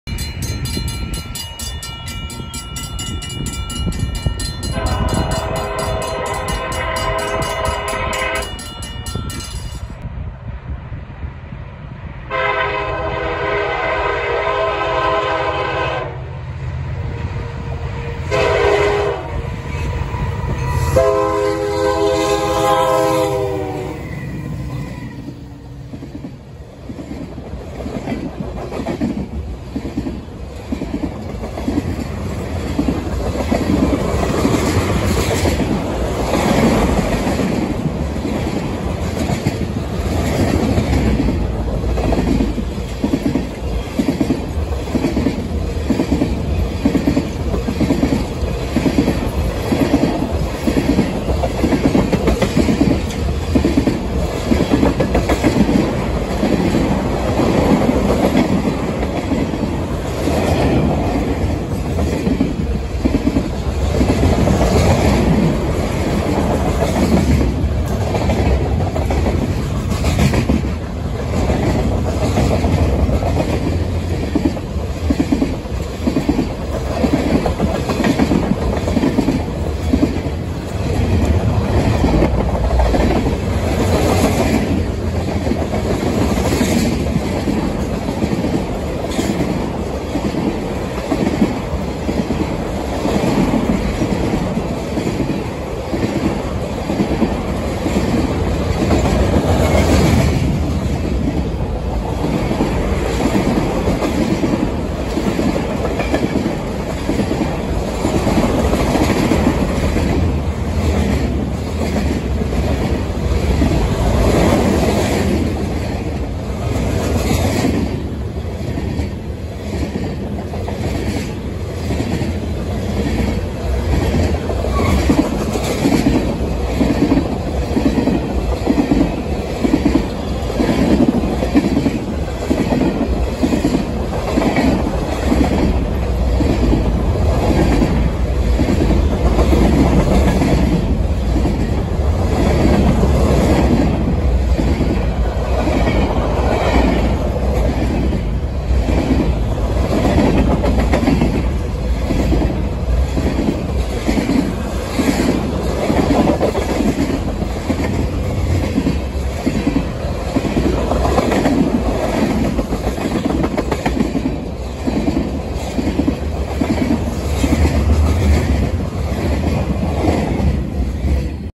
337 another wicked leader two days in row. Also all emd
337 on cn Freeport sub at Irene Illinois with up sd70m and cn sd75i on January 18 2022